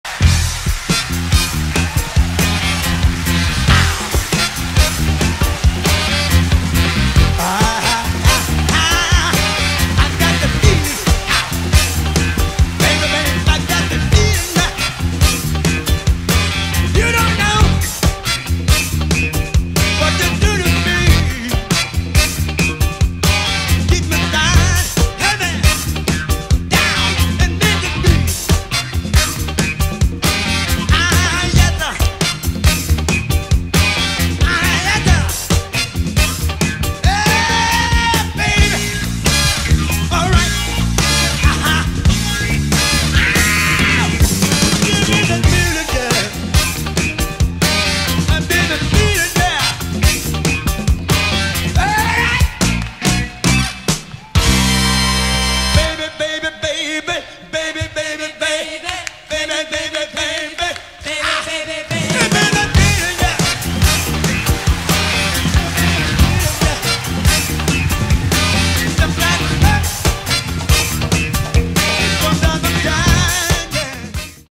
James Brown Groovin' On The 1
Let's take a trip to 79', Enjoy This Funky Dance Clip